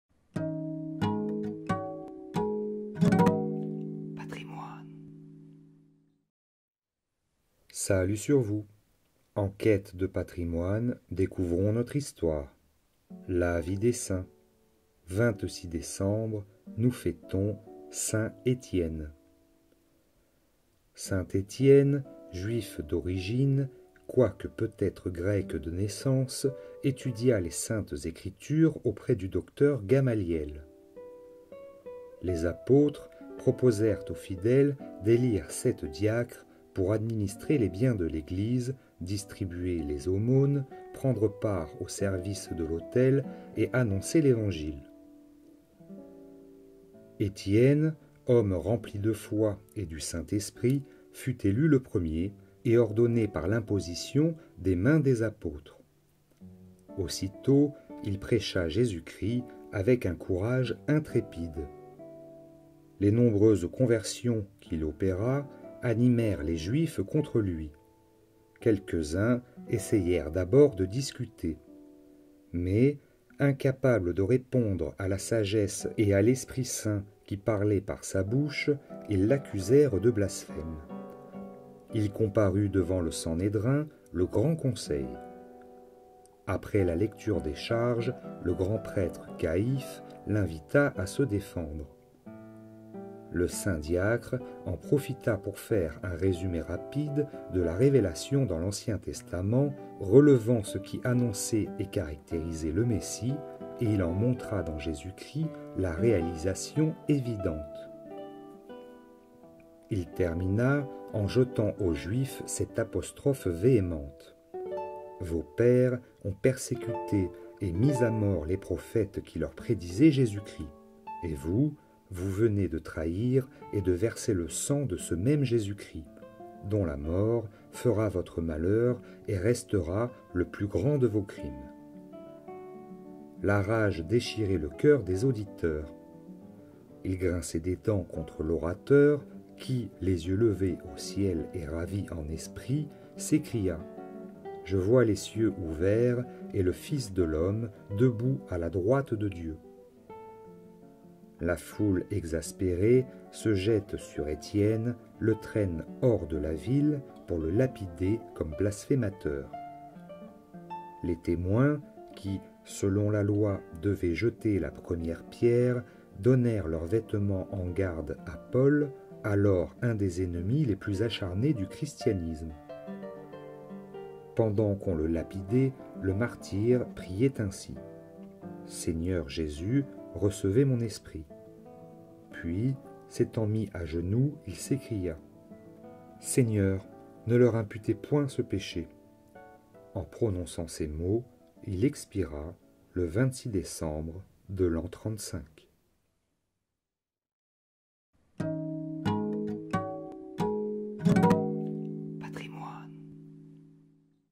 26 Décembre - Saint Étienne
Le saint du jour, d'après le livre "Vie des saints - Pour tous les jours de l'année" de Mgr Paul Guérin (Editions D.F.T.)